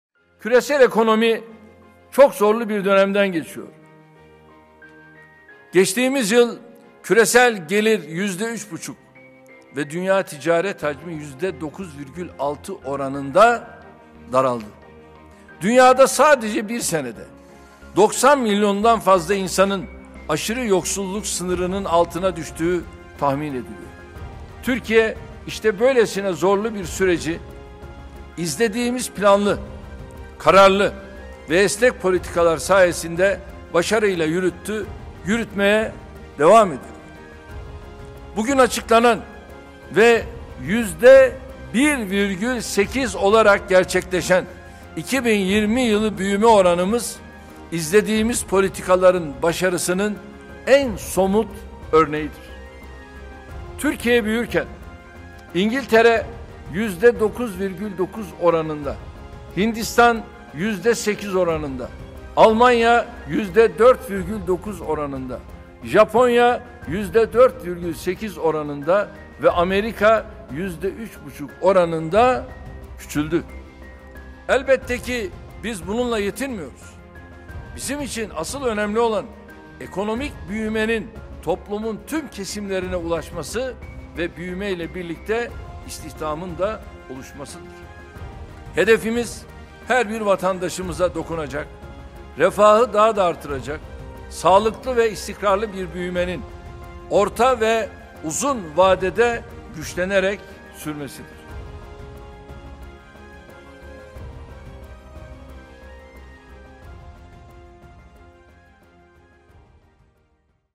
Hedefimiz Refahı Artıracak Bir Büyümenin Güçlenerek Sürmesidir
Bugün açıklanan ve yüzde 1,8 olarak gerçekleşen 2020 yılı büyüme oranımız, başarılı politikalarımızın en somut ifadesidir.

Hedefimiz, her bir vatandaşımıza dokunacak, refahı daha da artıracak sağlıklı ve istikrarlı bir büyümenin orta ve uzun vadede güçlenerek sürmesidir.